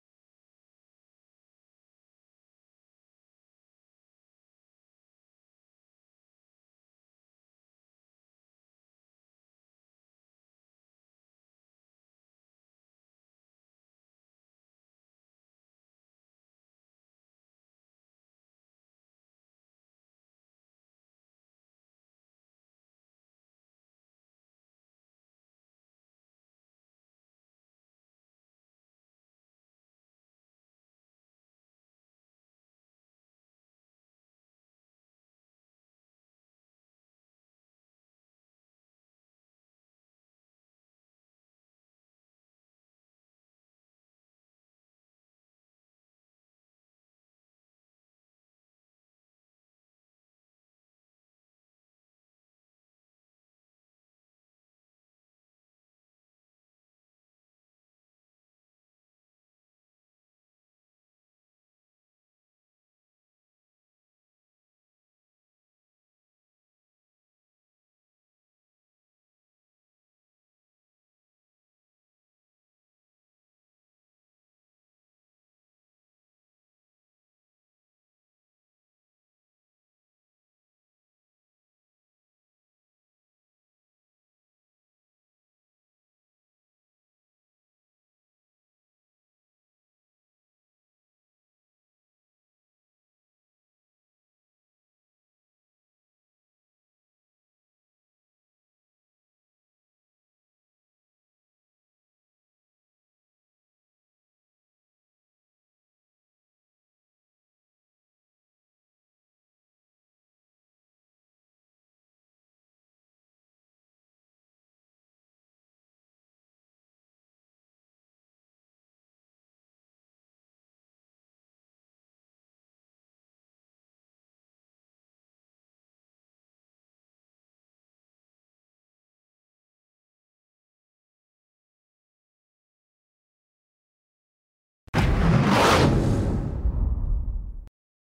Lamborghini Aventador 50° Anniversario - Shanghai 2013
Lamborghini a présenté une série spéciale de sa supercar lors du salon de Shanghai : la LP 720-4 50° Anniversario. Il s'agit d'une série limitée.

Si vous voulez réaliser votre rêve !! nous vous offrons un livre qui va changer votre vie